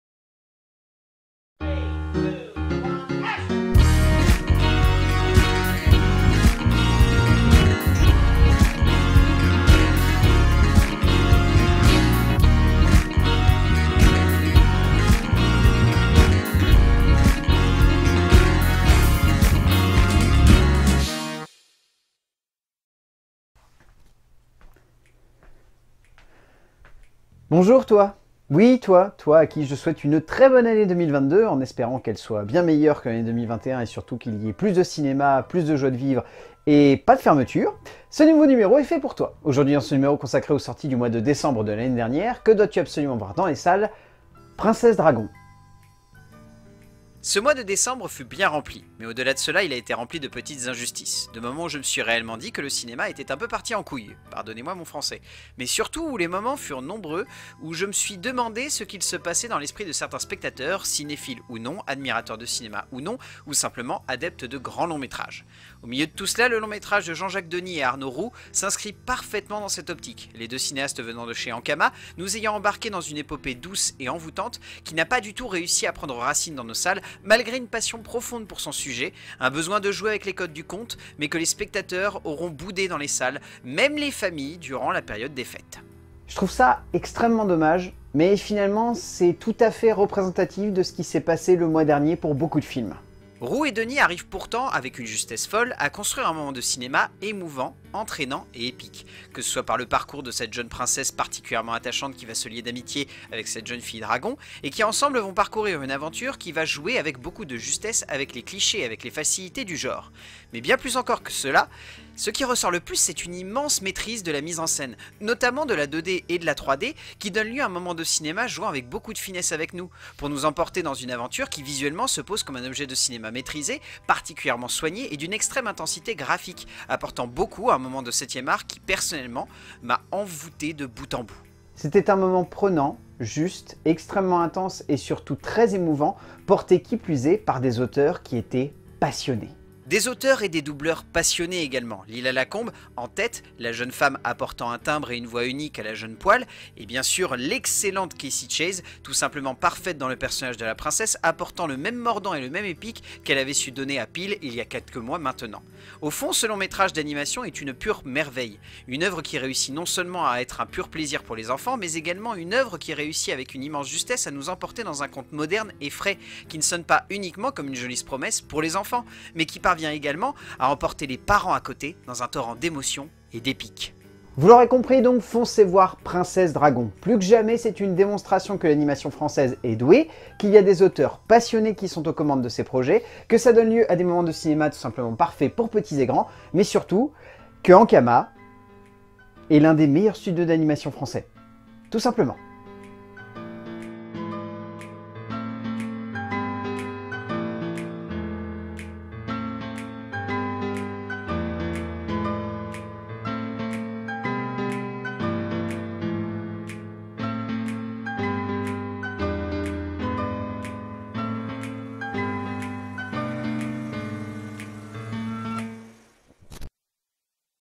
PTDELMV - Princesse Dragon (Décembre 2021)
Ce mois ci, nous allons nous attarder sur une petite pépite de cinéma d'animation à la française, une œuvre douce, délicate, entraînante et fun dans son genre, "Princesse Dragon"